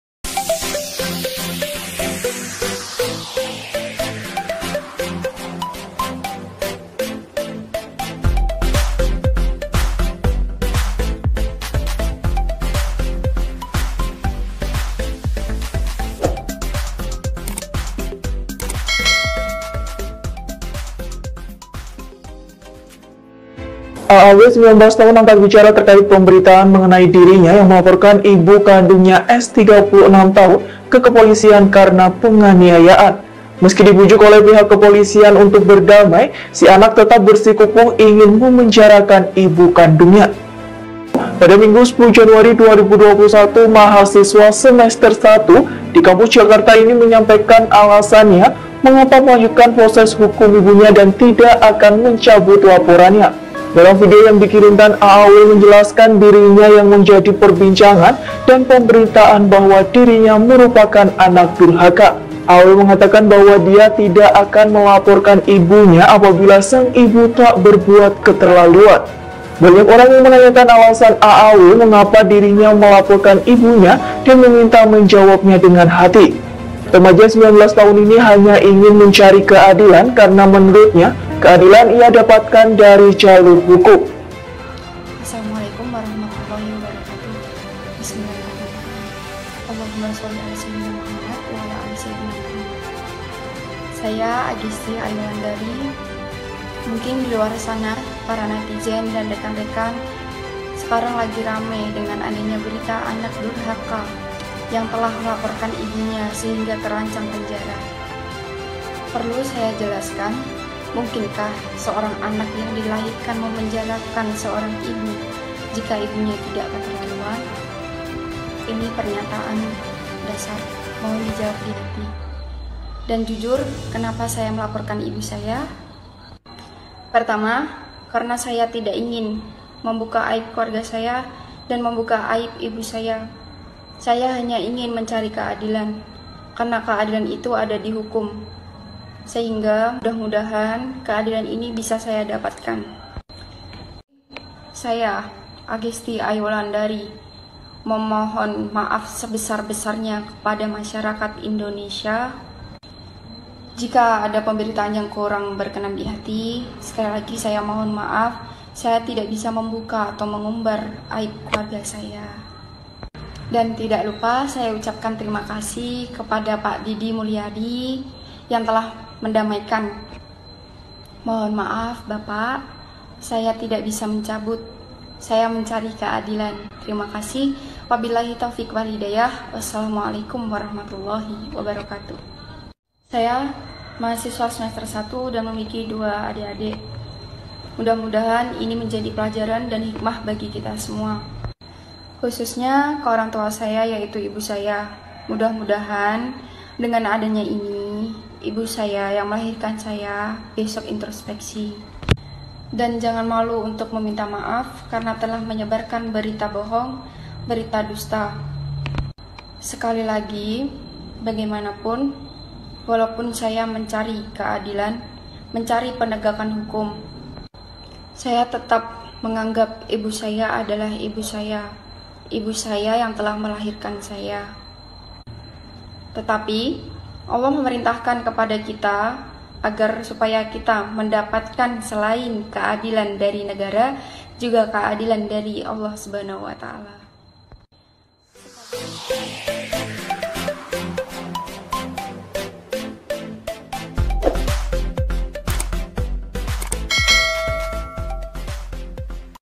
Inilah Alasan Agesti Ayu Mau Penjarakan Ibunya
Agesti Ayu Wulandari (19) angkat bicara terkait pemberitaan mengenai dirinya yang melaporkan ibu kandungya Sumiyatun (36) ke kepolisian karena penganiayaan